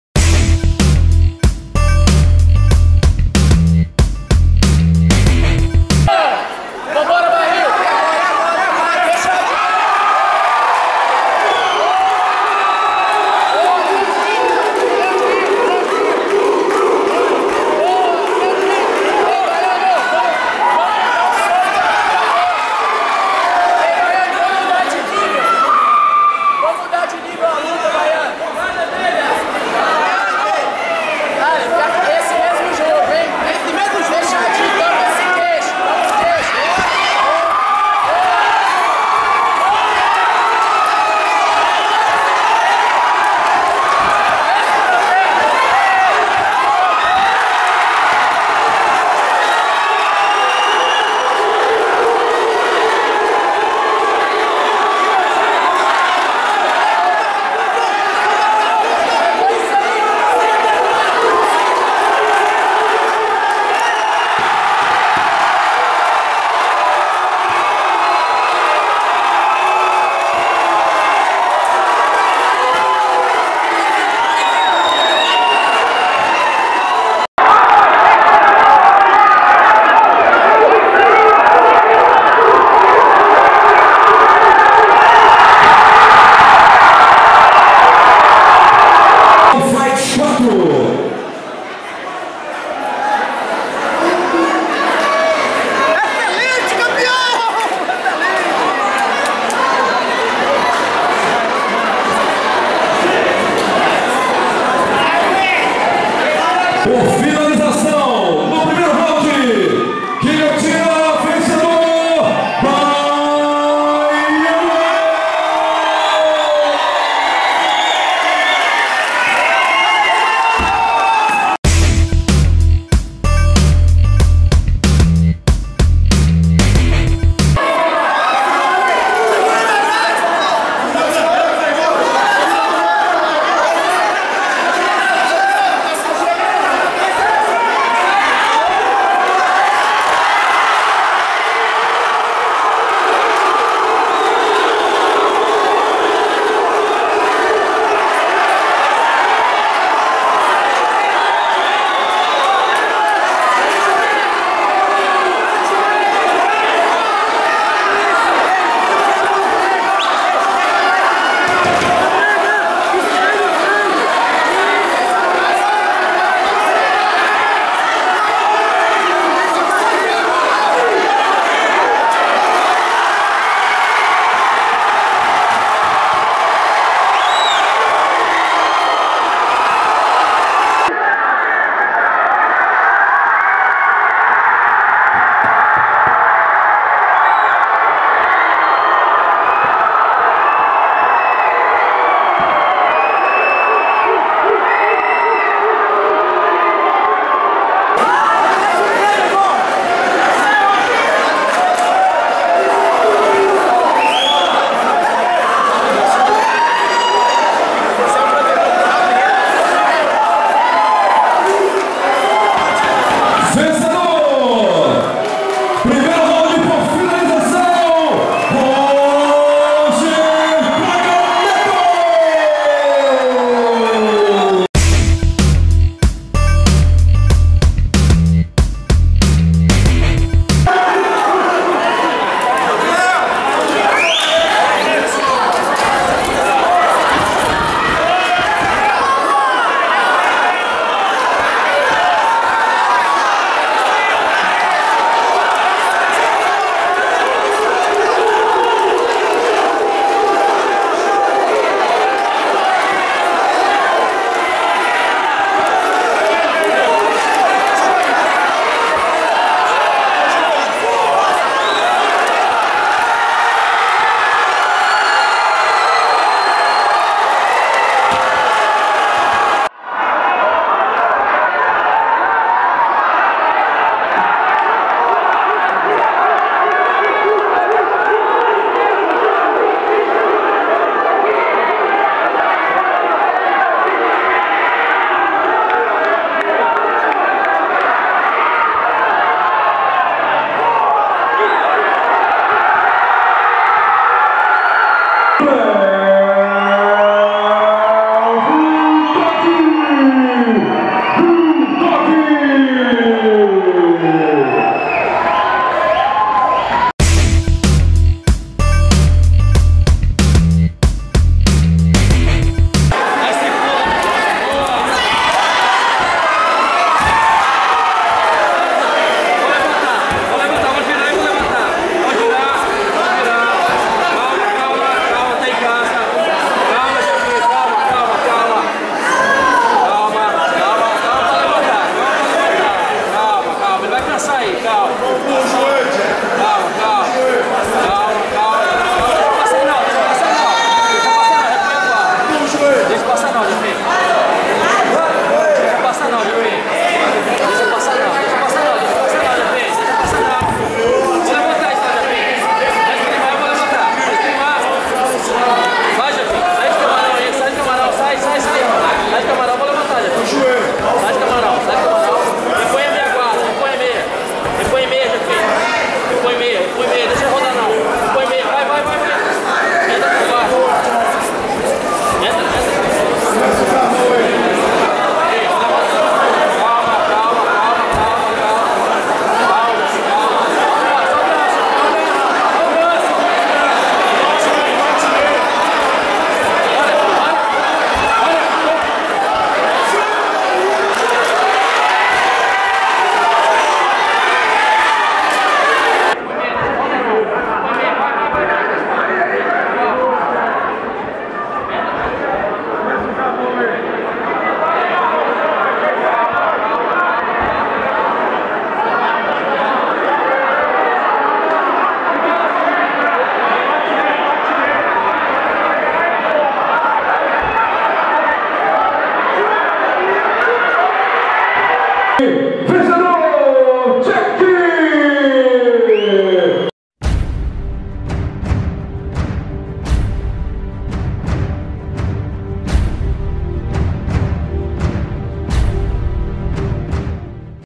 Melhores finalizações One vs One 4
Hoje é dia de especial com as melhores finalizações do One vs One 4, evento de MMA realizado em Teófilo Otoni, em setembro de 2014. O primeiro lugar ficou com Eduardo Juneo com uma belíssima kimura, após levar o combate para o chão. De qual evento você gostaria de ver as melhores finalizações e nocautes?

Top 1
Eduardo Juneo Jackie (TFT/ Montes Claros) venceu por finalização aos 1:30 do primeiro round Jefferson Mulford (Team Maguila/ Espírito Santo).
Top 2
Leandro Bulldog (TFT/ Teó₢filo Otoni) venceu por finalização aos 0:28 do primeiro round João Paulo (Thai Boxe Team/ Montes Claros).
Top 3
Roger Braga Neto (TFT/ Teófilo Otoni) venceu por finalização aos 1:58 do primeiro round Carlos Alberto Souza "Carlão Trator" (Team Marreta/ Porto Seguro).
Top 4
Alef Andrade "Baiano" (TFT/ Teófilo Otoni) venceu por finalização aos 1:51 do primeiro round Carlos Vieira "Carlinhos Relâmpago" (Gracie Barra/ Montes Claros).